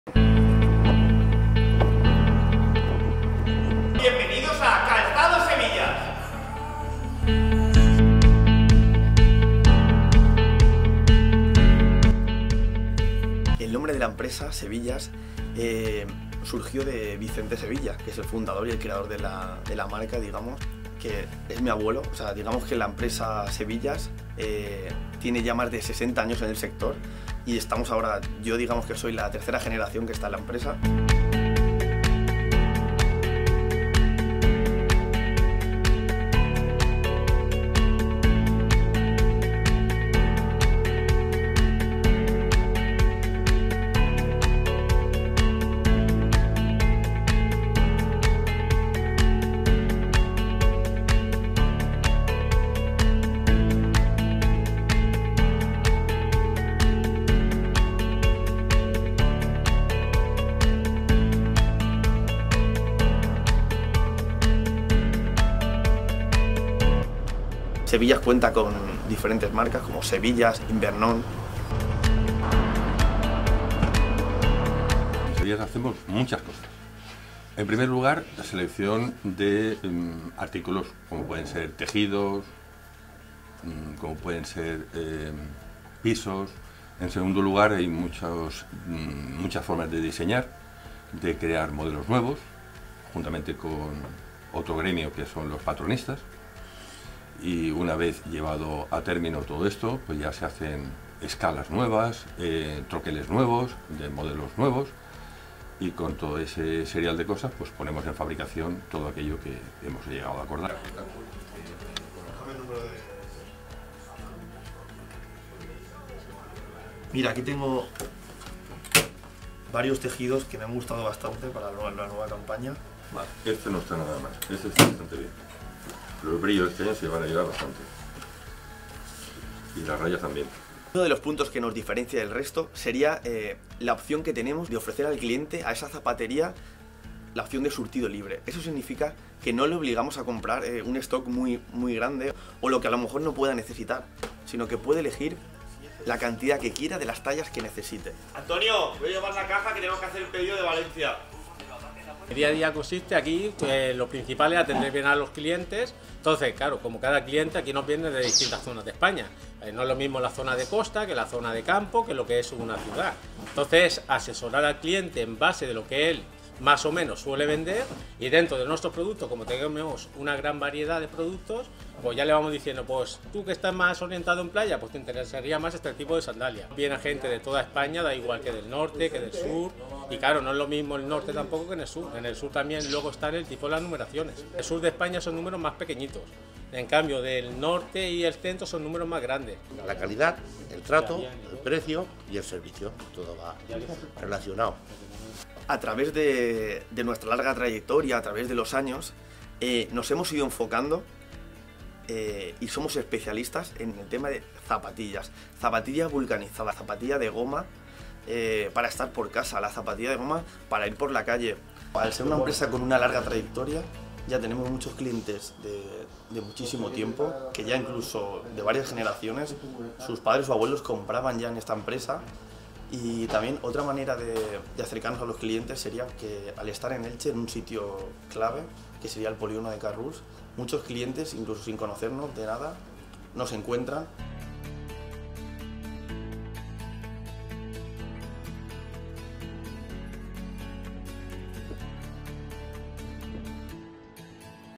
Calzado al por mayor España SEVILLAS (Fabricación y Mayorista)
Sevillas es un empresa con una trayectoria en el sector de más de 60 años de experiencia, que se encarga del diseño, fabricación y distribución de calzado al por mayor, siendo proveedor de calzado para muchísimas zapaterias en toda España.

Somos especialistas en calzado tanto para señora, caballero como niño, fabricados íntegramente en España y fundamentalmente nuestra especialidad se centra en alpargatas con diseños y zapatillas de estar por casa.

Podrás comprar calzado al por mayor en nuestro almacén en Elche, en surtido libre, es decir podrás comprar pares sueltos, del modelo que necesites, sin ningún tipo de mínimos. Hacemos envíos a todo el mundo.
Nuestro equipo está en disposición para atender y asesorarte de forma totalmente desinteresada en todo aquello que necesites relacionado con este tipo de artículo.